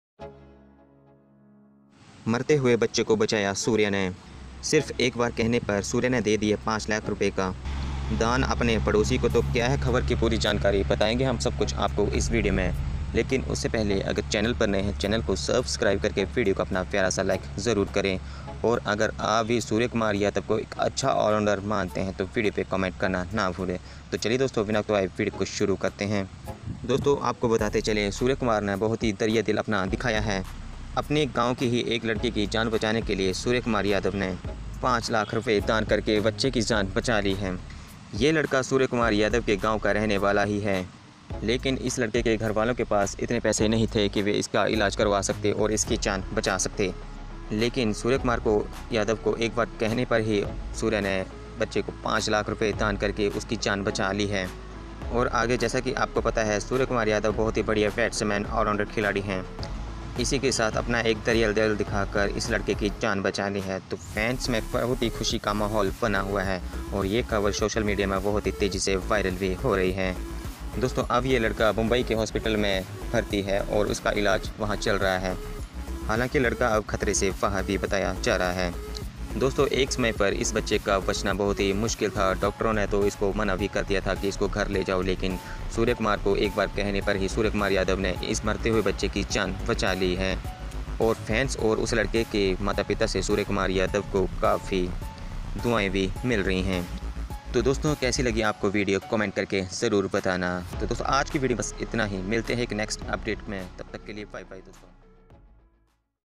सिर्फ एक बार कहने पर सूर्या ने दिया 5 लाख रूपये दान। Surya Kumar Yadav
¤My Instagram Id Link Here